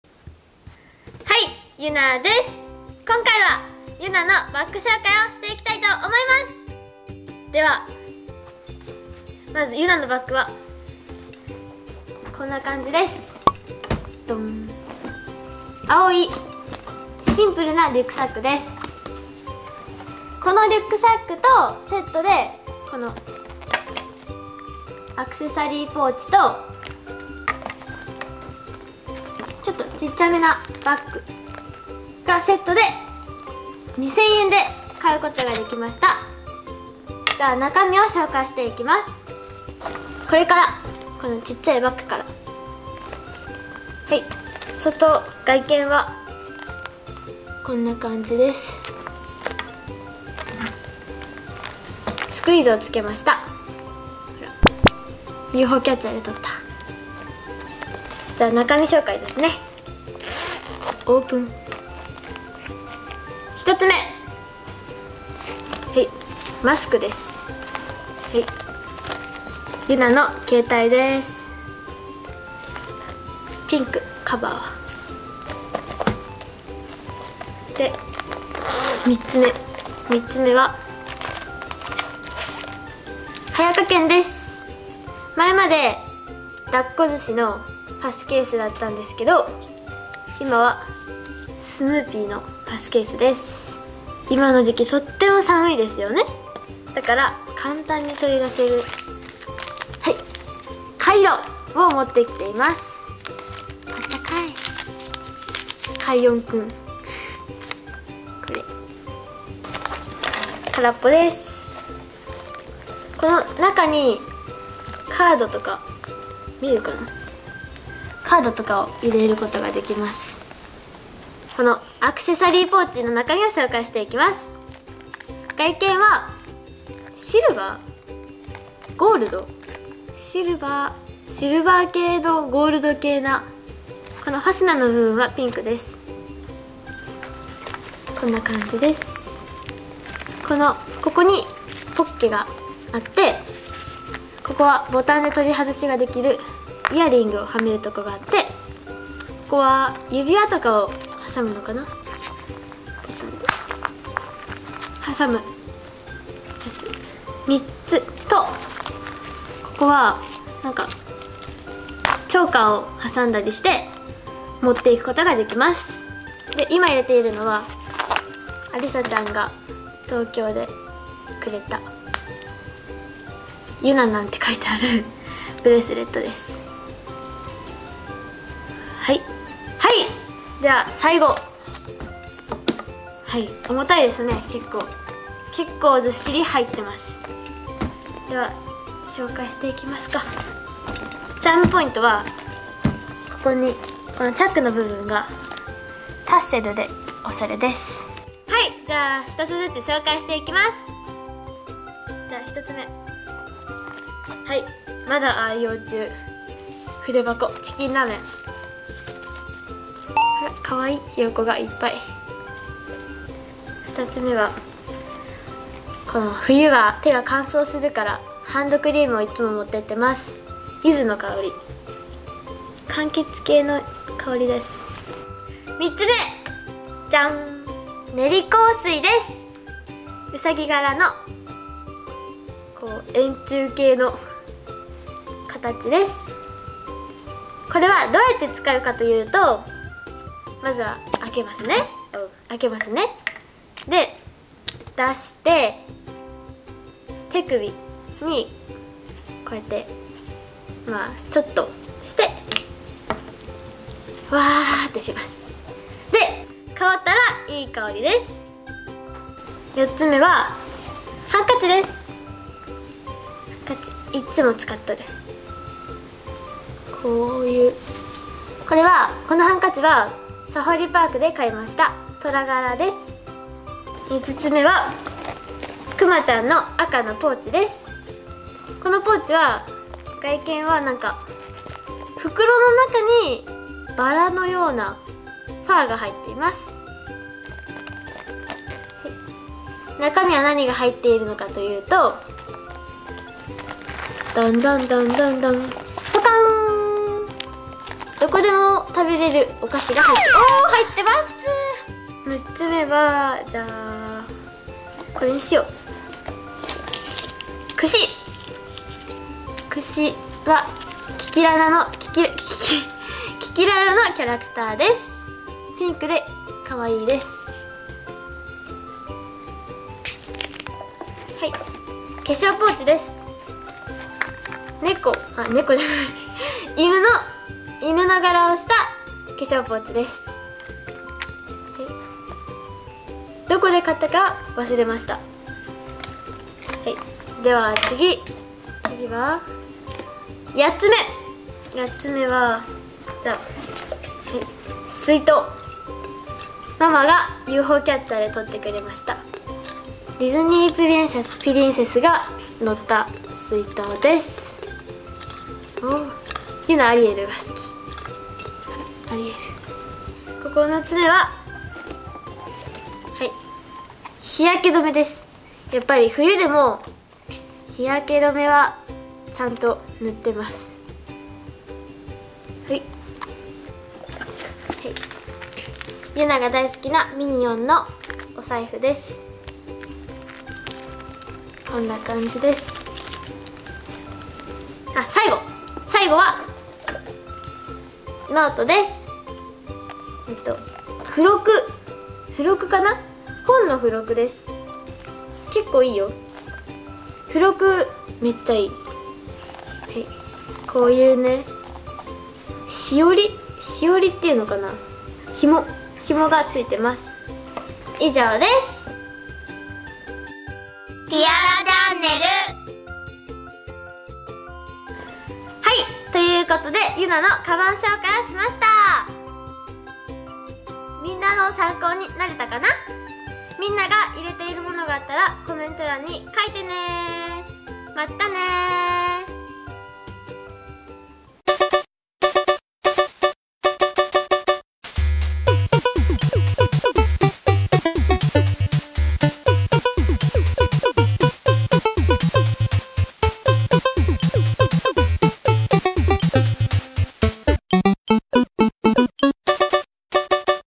超お買い得！ゆなのカバンの中身紹介♡
今回はゆなが冬のお出かけ用のカバン紹介＆中身を紹介してくれたよ！

【Twitter】→@onemoacyan
【Tik Tok】→tiaraprot